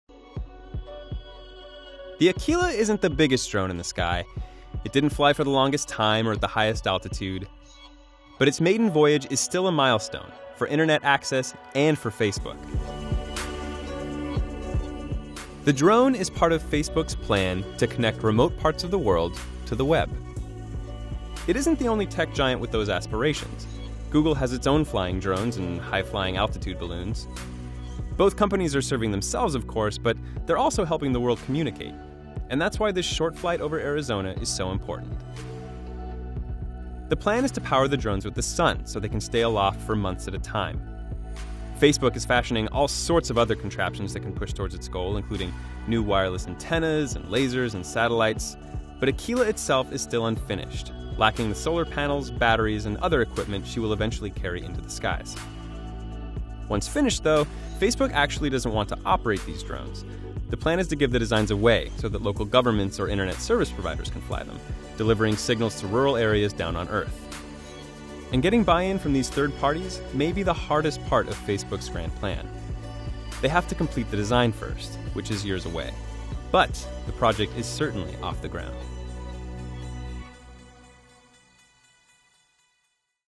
Facebook's Huge Internet-Beaming Drone Takes Flight | WIRED
Facebook’s huge internet-beaming drone finally takes it's first flight and it's all part of the tech giant's plans to provide internet access in remote parts of the world.